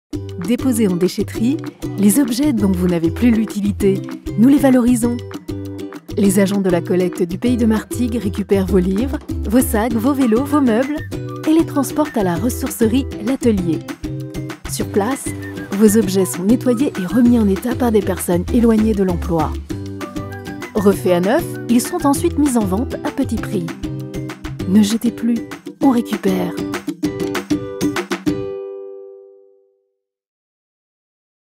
Ressourcerie du Conseil de Territoire
Avant de jeter, pensez ressourcerie !
L’Atelier récupère les meubles, petits électroménagers, multimédia, vaisselle,
bibelots... pour leur donner une seconde vie !
Rendez-vous 5 rue Paul Painlevé à Croix-Sainte, Martigues.